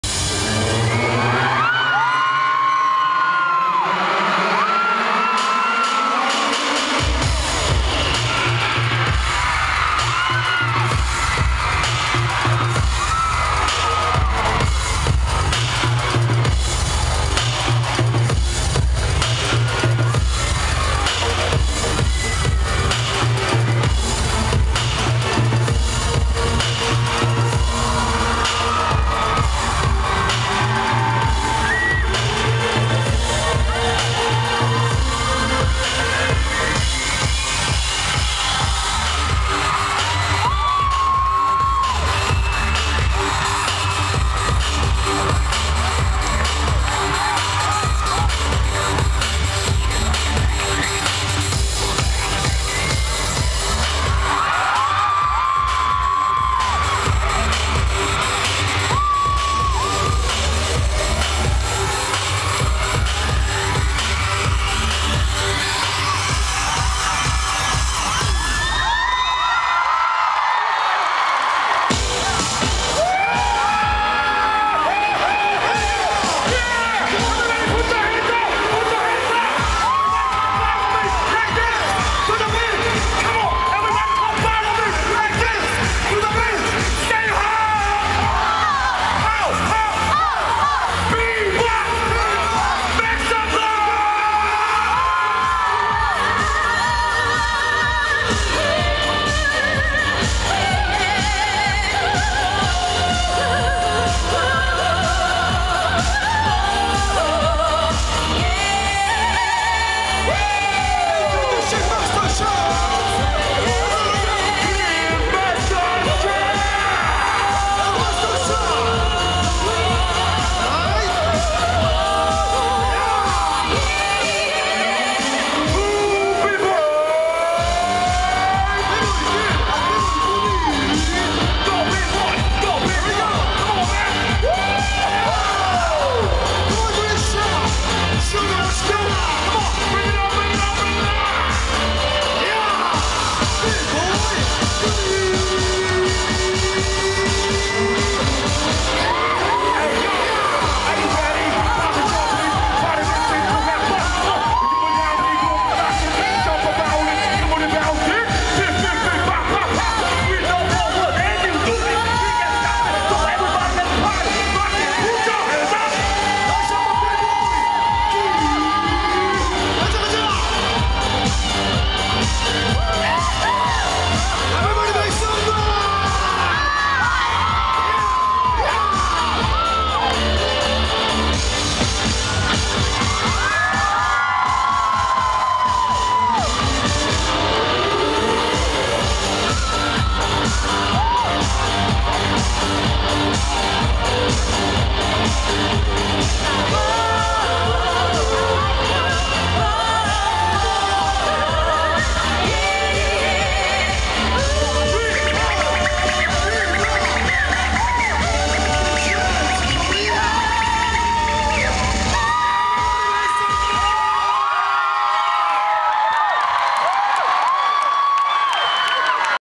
[MLIVE by CJ MO.A 2012 in VN] 24/11/2012 - BiBap
Chương trình MLIVE MO.A in VN ngày 24/11/2012. Chất lượng hình ảnh và âm thanh có thể ko tốt mong mọi ng thông cảm. Không khí sôi động mọi ng đều nhảy và cổ vũ nên ko nghe rõ.